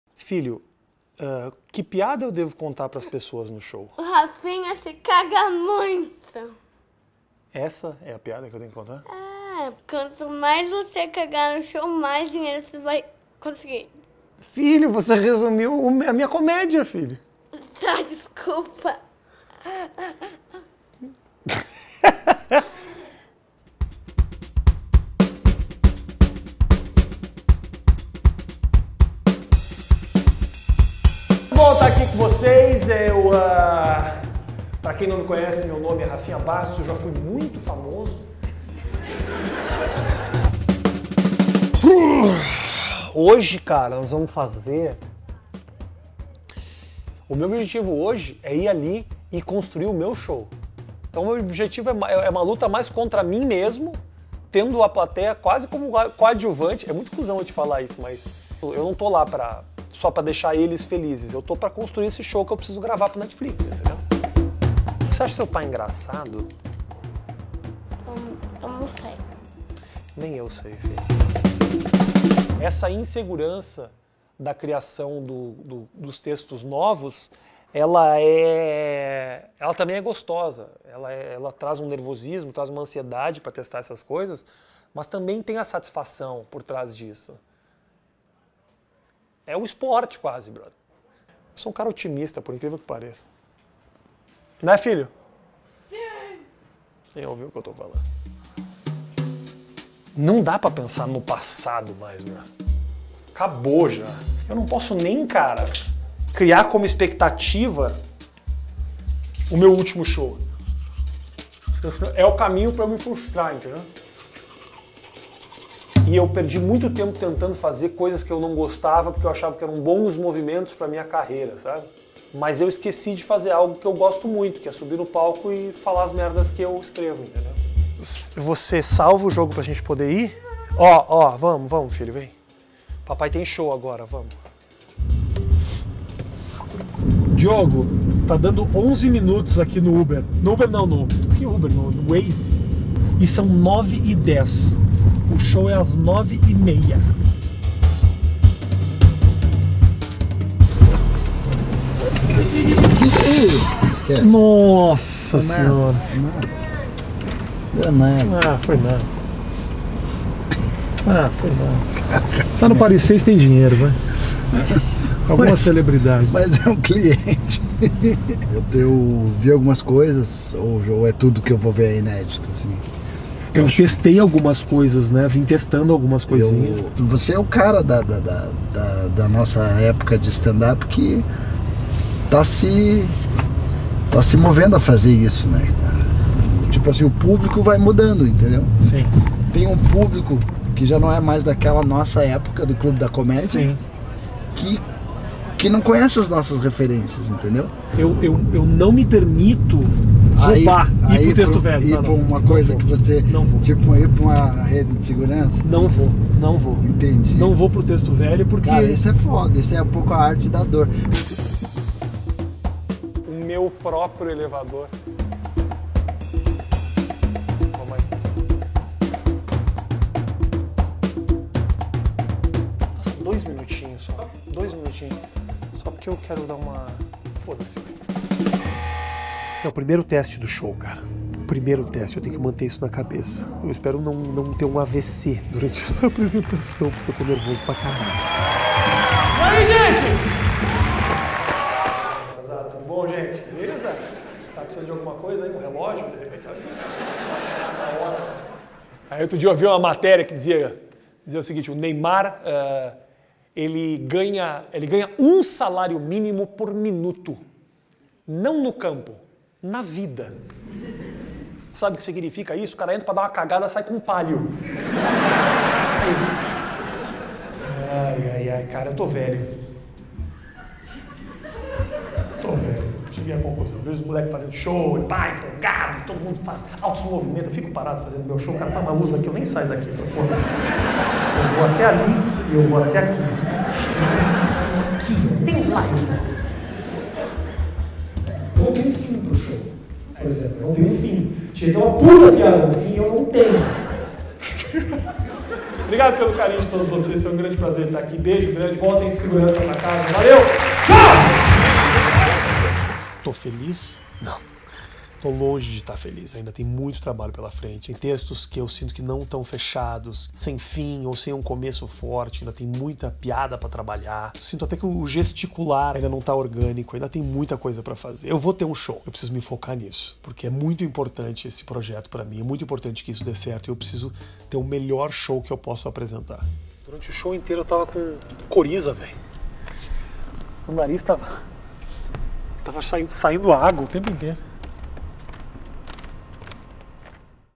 POR TRÁS DO SHOW (2/4)
O show estréia dia 21/9 (sexta que vem!!!). Nessa série você conhece os bastidores de como ele foi criado.